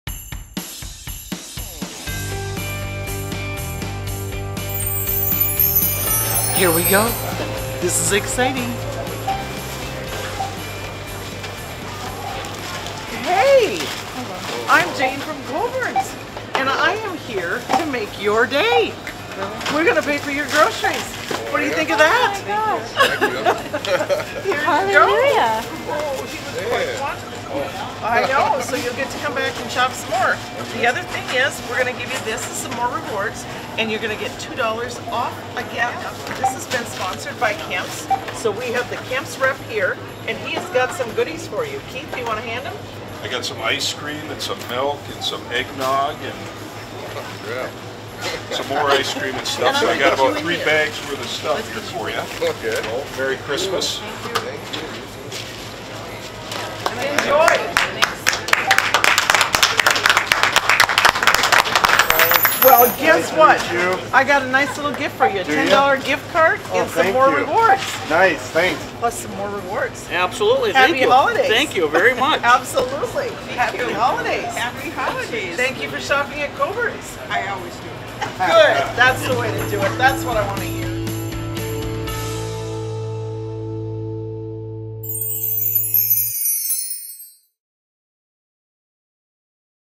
Little Falls, MN - The MORE the Merrier
We're buying your gas & groceries in Little Falls, MN thanks to Kemps! YOU COULD BE NEXT! for details.